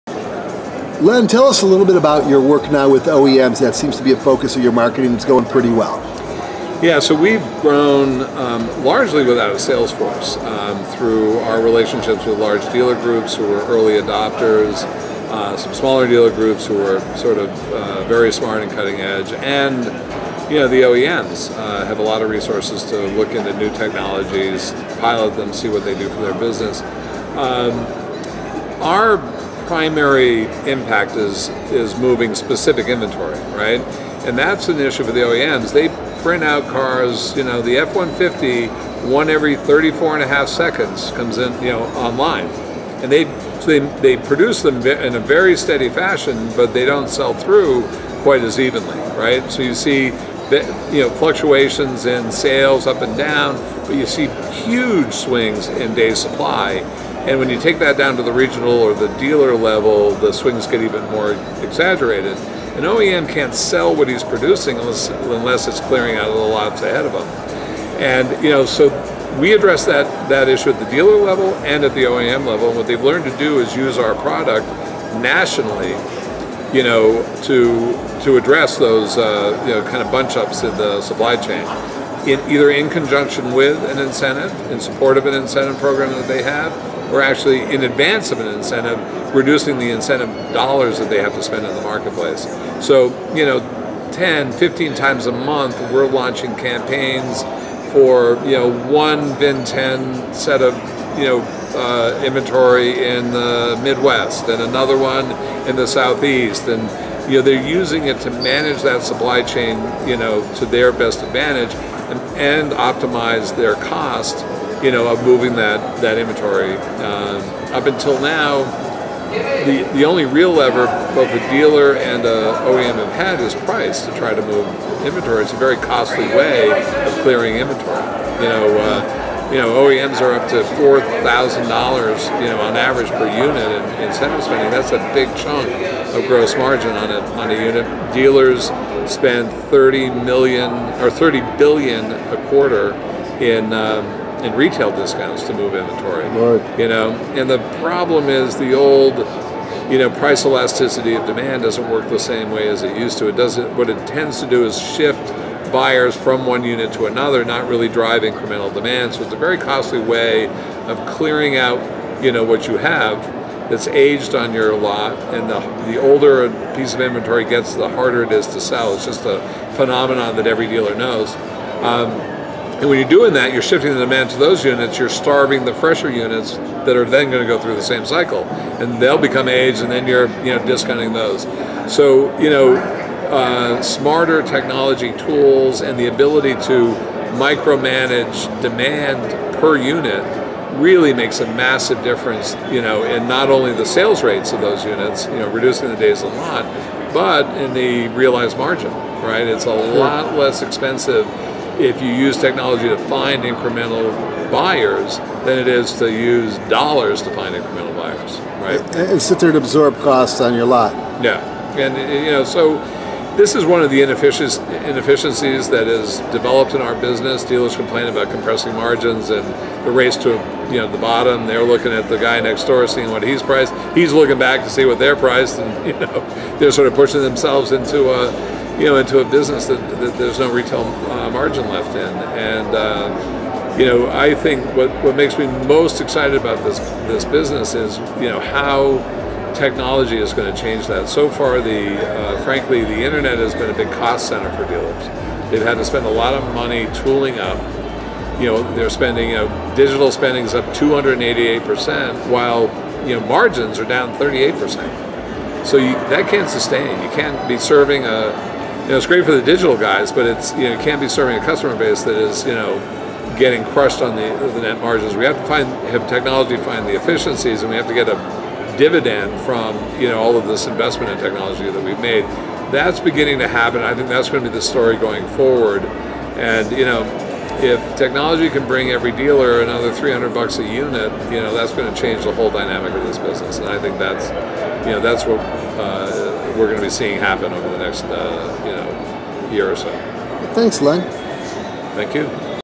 Dealership News: LotLinx Interview with Len Short Pt2
Dealership News' Kelly Kleinman continues his interview with Len Short of LotLinx to find out how the company helps O/E.M.'s with their unique issues.